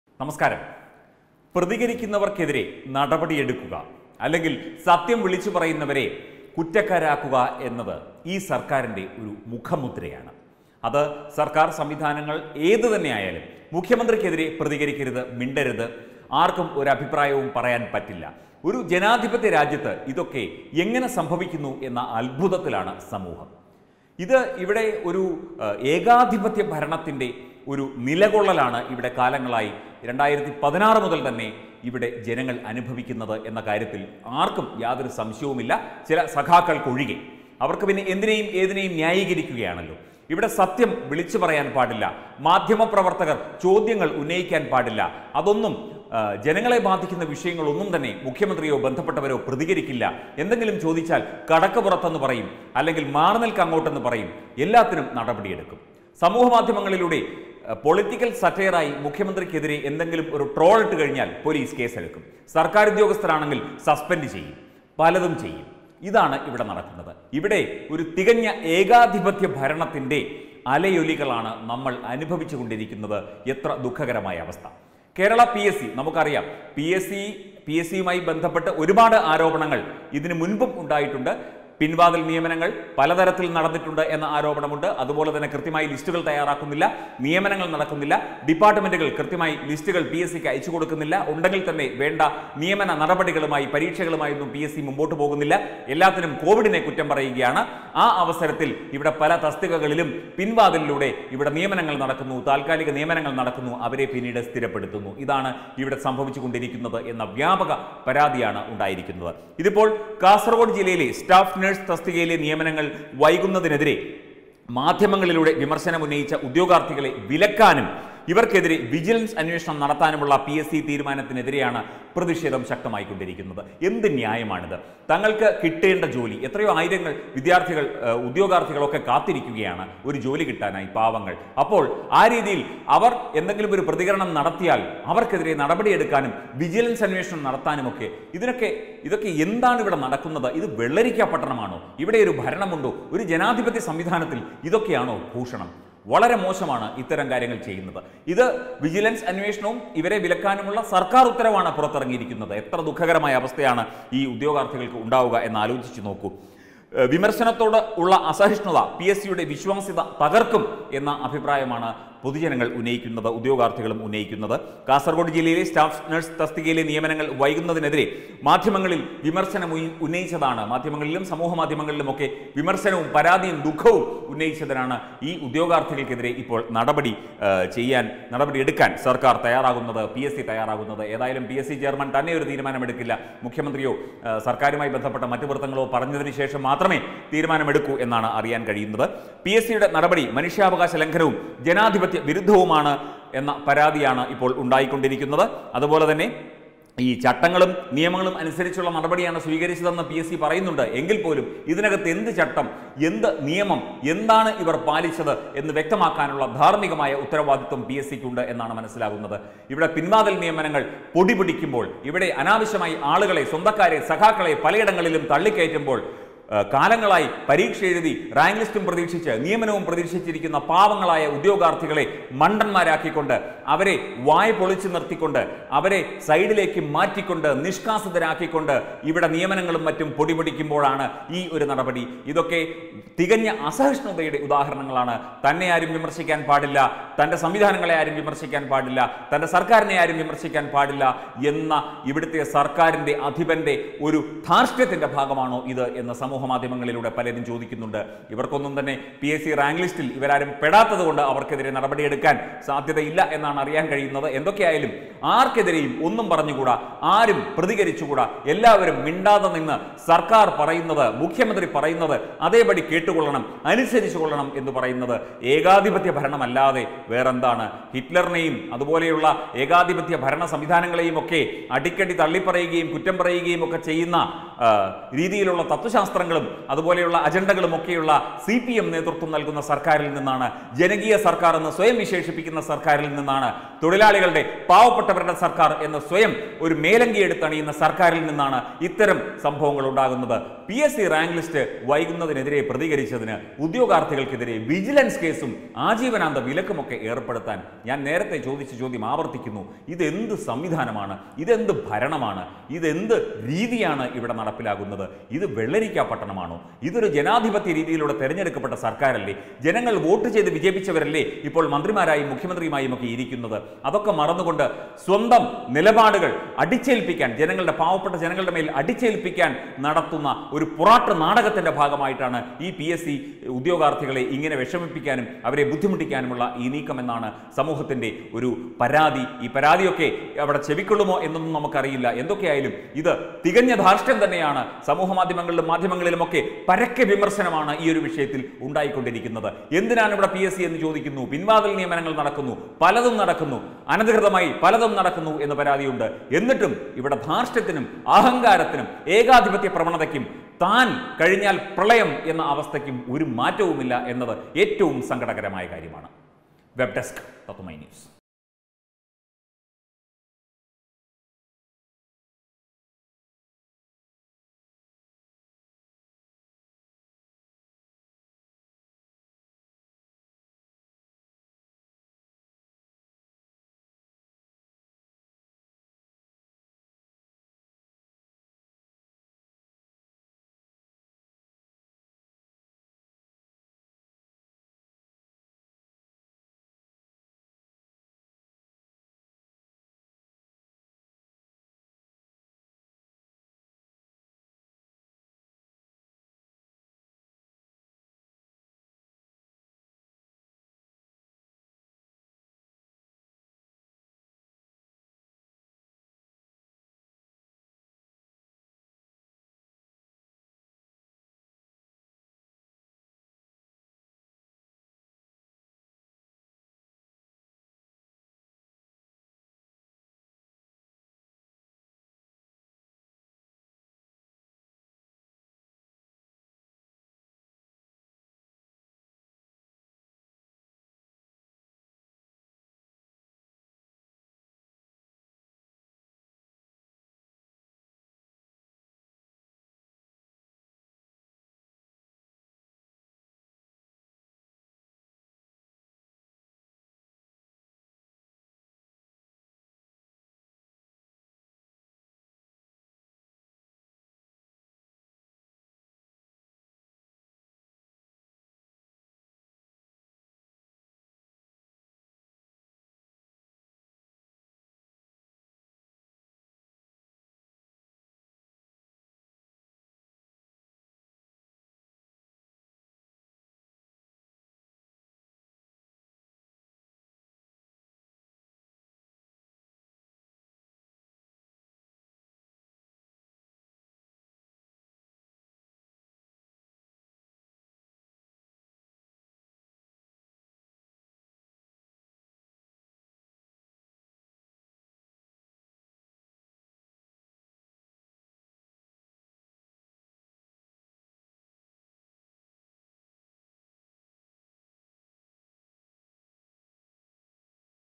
PSC യെ ക്കുറിച്ച് എന്തെങ്കിലും മിണ്ടിയാൽ, ആജീവനാന്ത വിലക്ക് + വിജിലൻസ് കേസ്
PSC യെ ക്കുറിച്ച് എന്തെങ്കിലും മിണ്ടിയാൽ, ആജീവനാന്ത വിലക്ക് + വിജിലൻസ് കേസ്psc